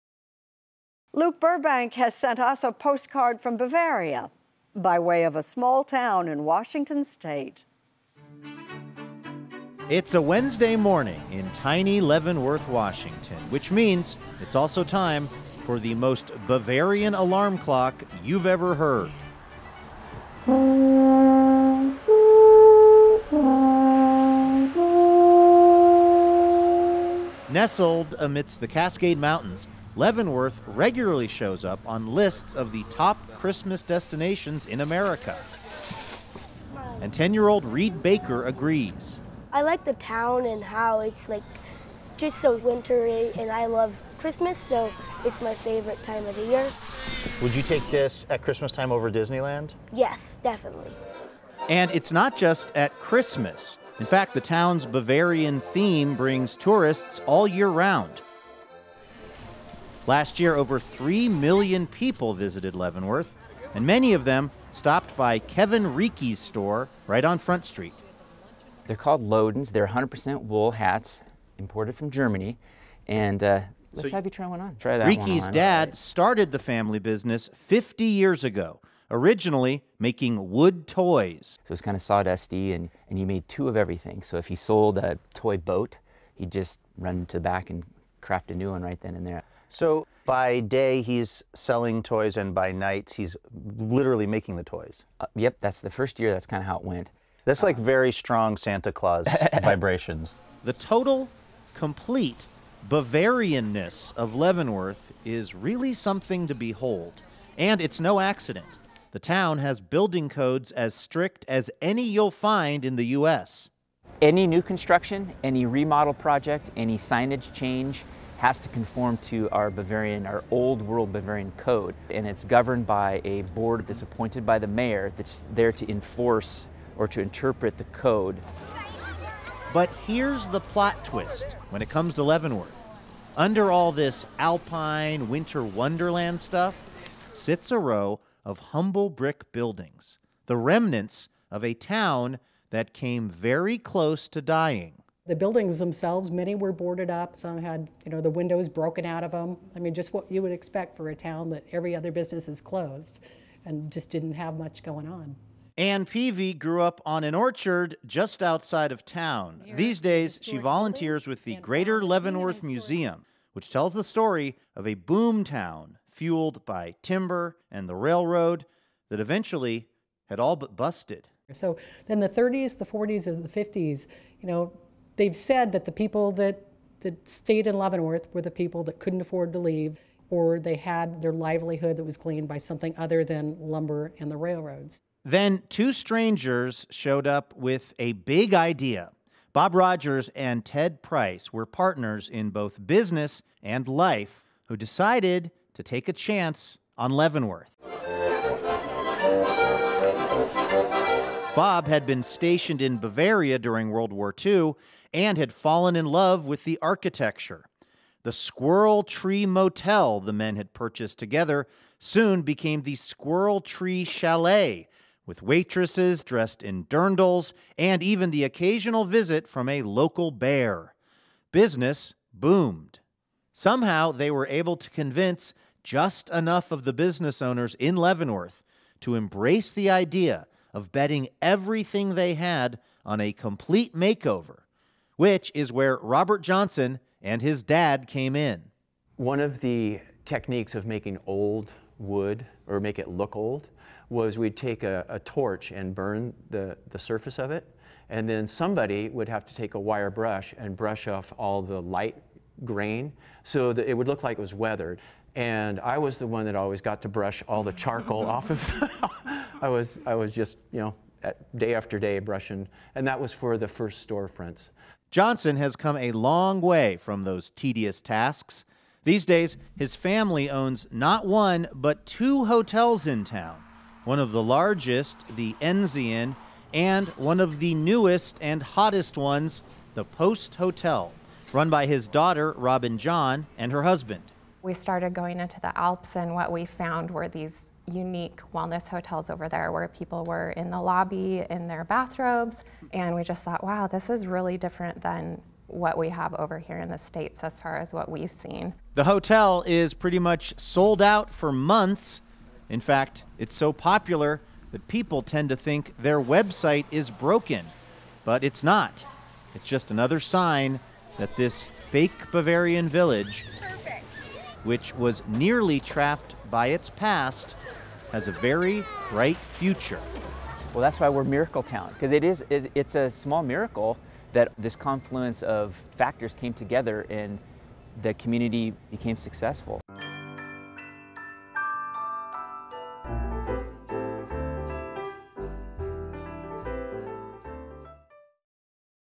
"Willkommen" to Washington State's own Bavarian village
Nestled amidst the Cascade Mountains in Washington State, Leavenworth historically didn't have a strong connection to the Old World. But after the railroad re-routed and the lumber mill closed, the tiny town began a process of "Bavarianization" to lure tourists to its alpine scenery. Today the place regularly shows up on lists of top Christmas destinations in America, even drawing millions of tourists all year long. Correspondent Luke Burbank examines how a former boom town gone bust remade itself as a German mountain village.